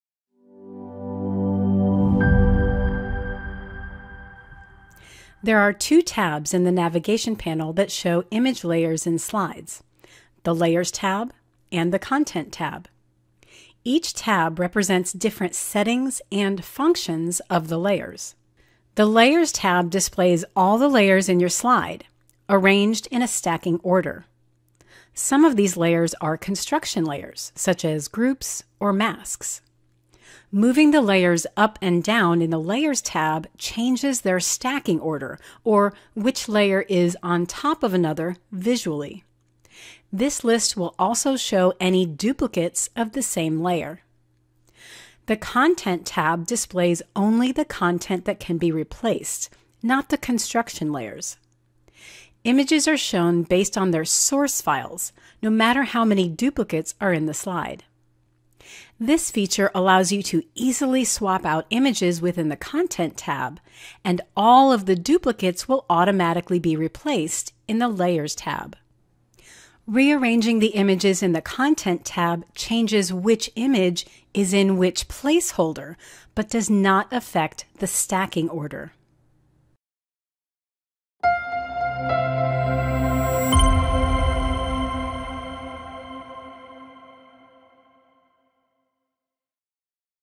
Essentials Guide to the Layers and Content Tabs
In Photopia, the Layers and Content Tab may appear similar at first glance, but they have distinct functions in slide design. This tutorial will explain the differences between these two tabs and guide you on when to use each one.